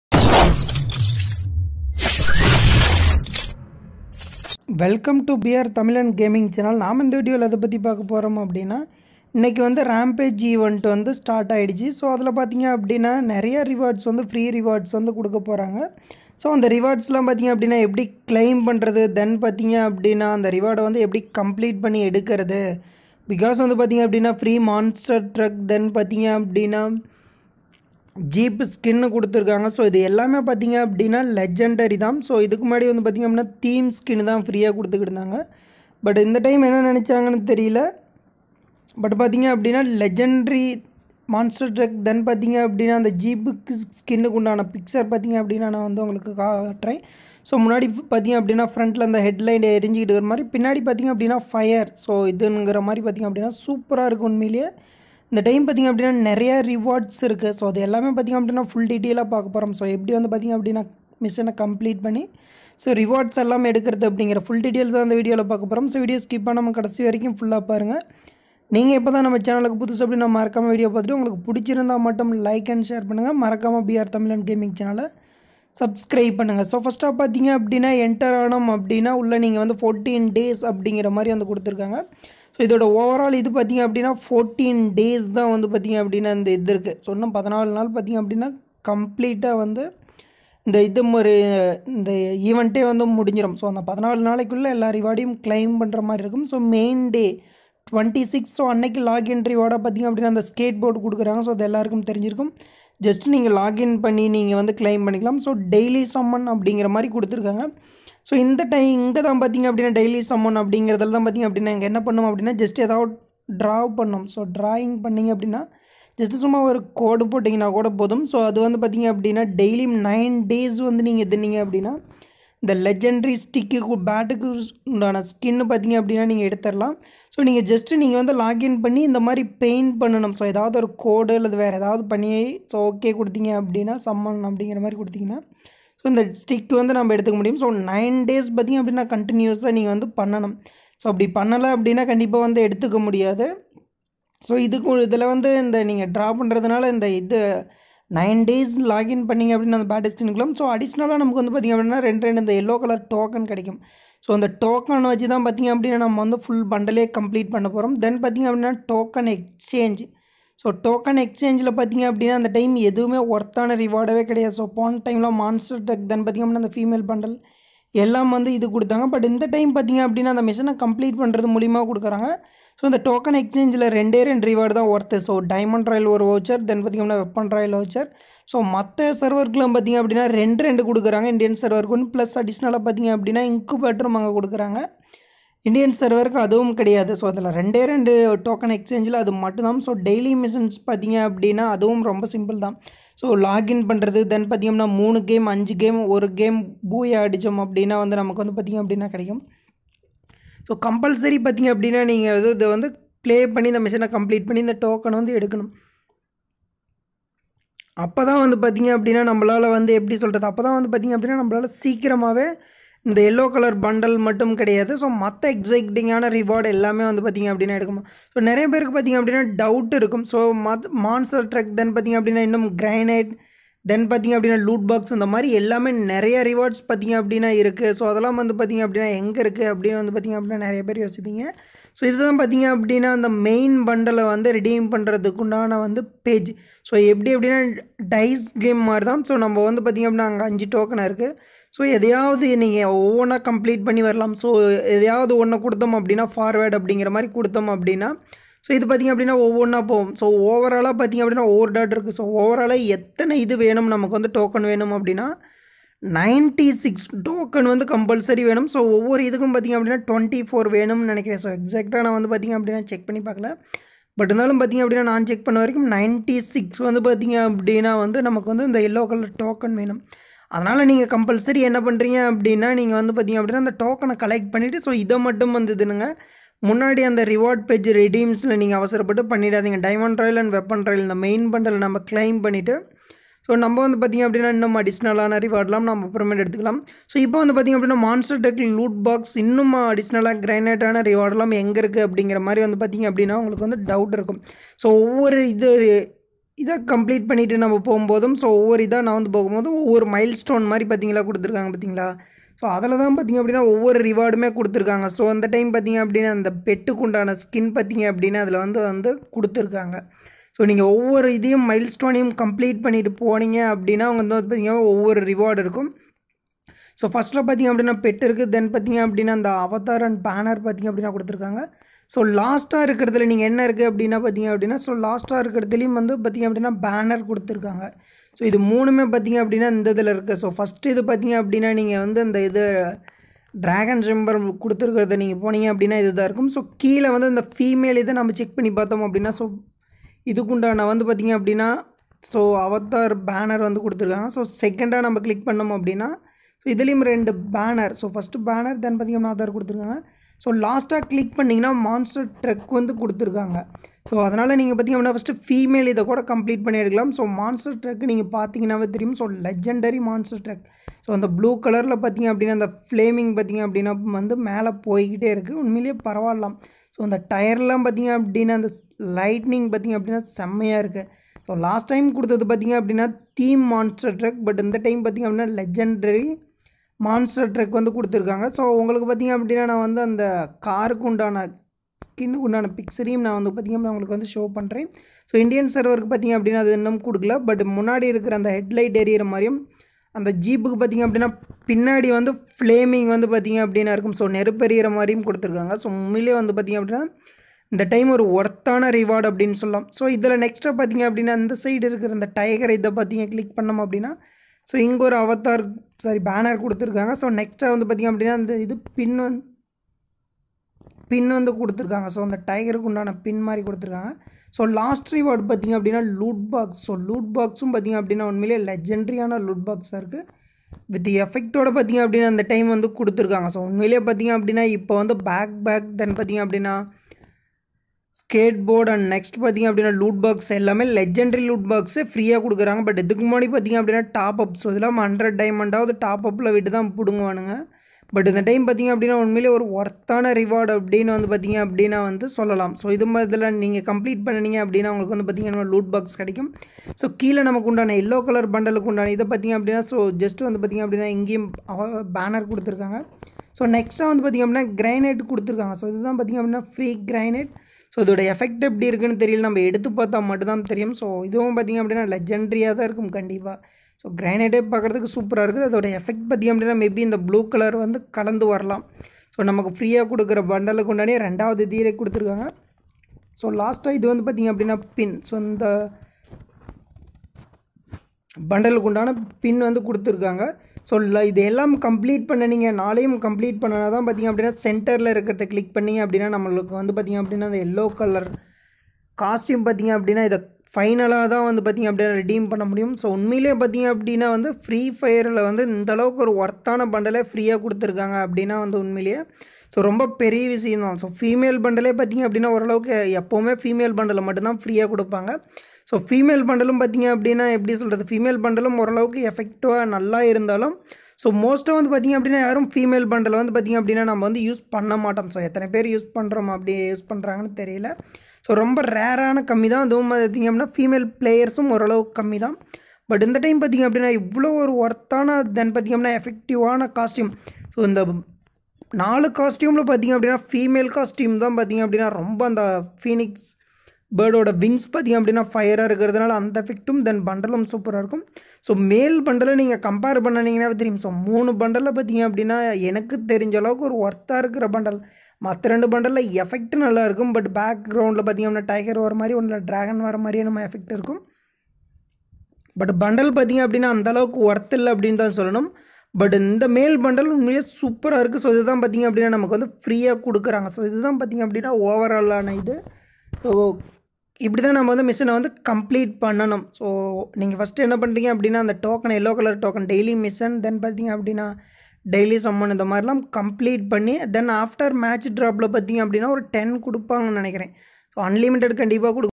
Rampage 3.0 event | Rampage event free rewards claim details | BRT GAMING
How to complete Rampage event in Tamil